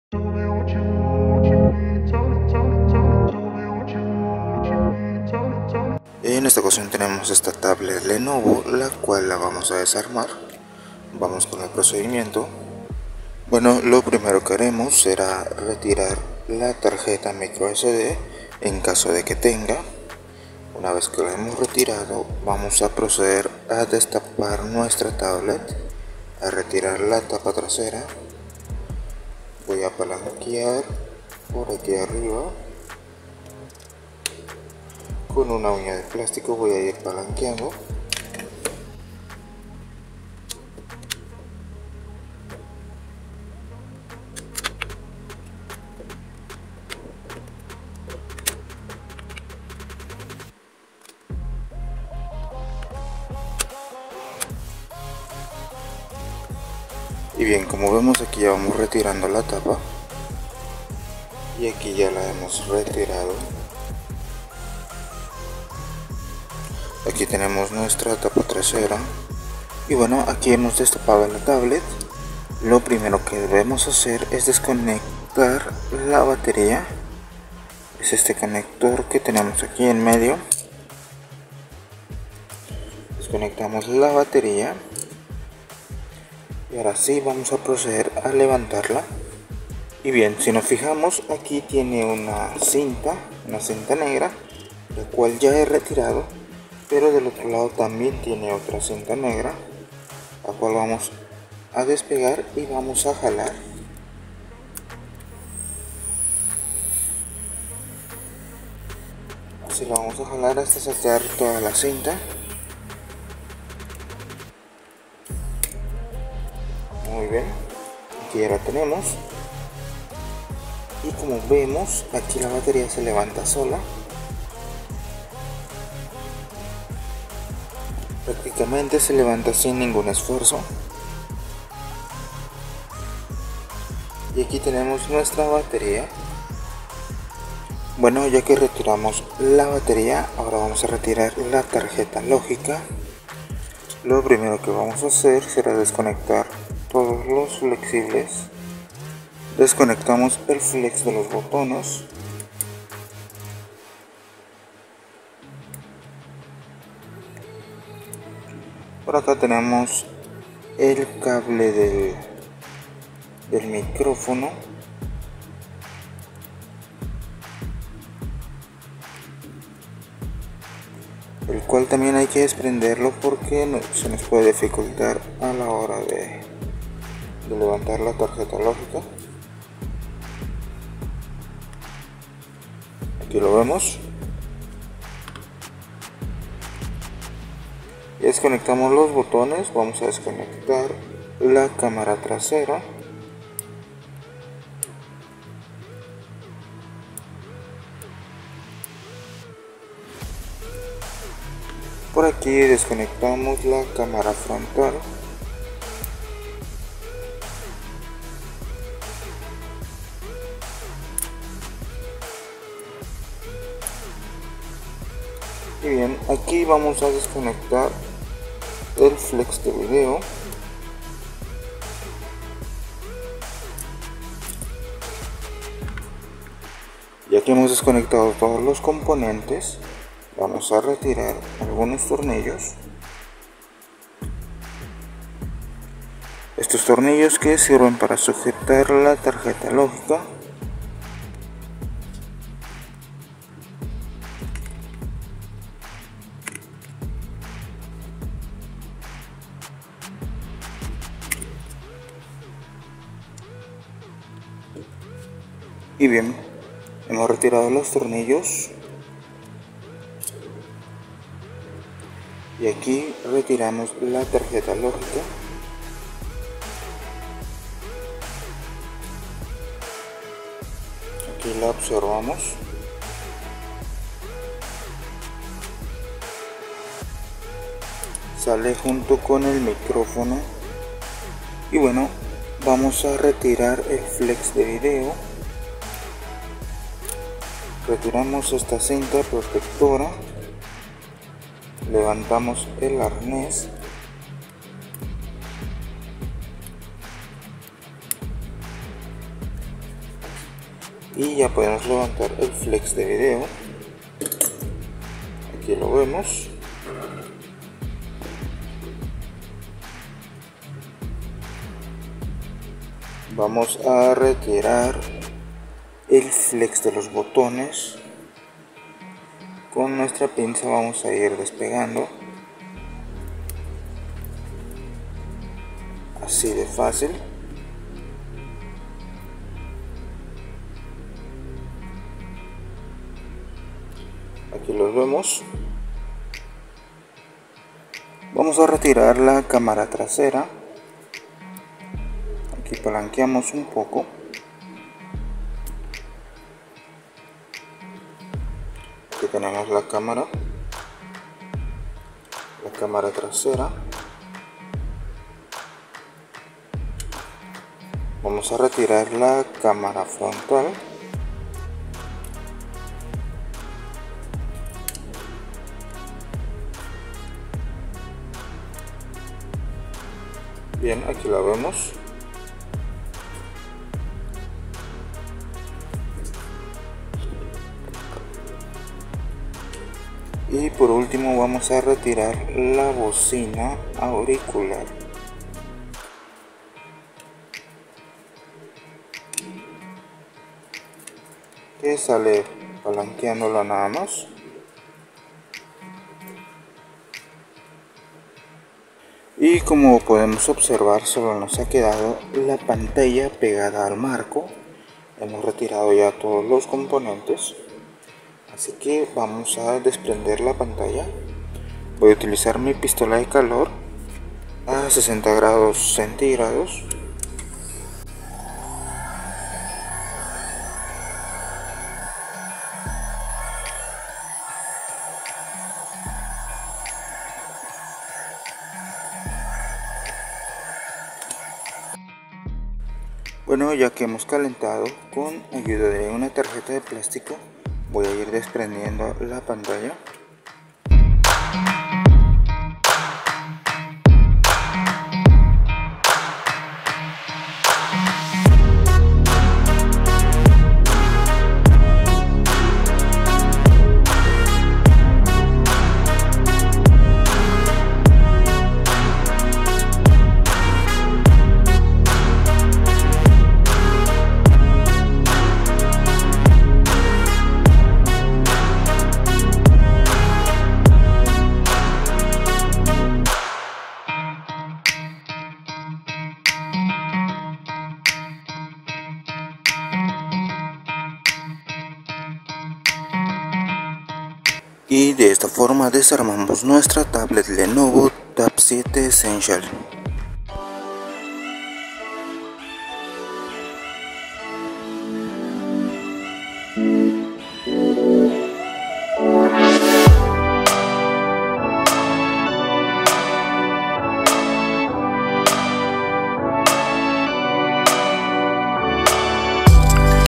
Còmo desarmar tablet Lenovo Tab 3 7 Essential
En èste vìdeo te muestro como puedes desarmar tu tablet lenovo paso a paso.